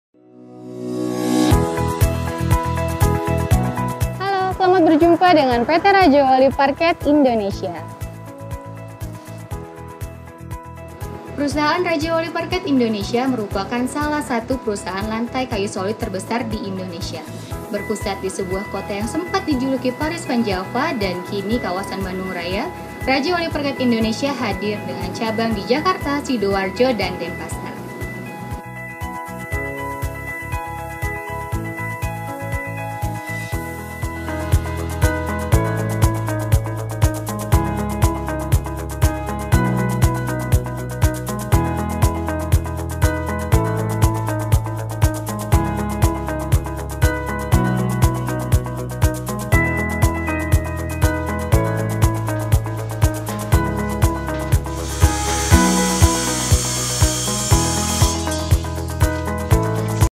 RAJAWALI PARQUET
Rajawali patquet
Produk Lantai Kayu berkualitas
. Good Quality
. Kuat dan tahan lama
. Tidak dimakan rayap
. Elegan dan mewah
. Harga sangat terjangkau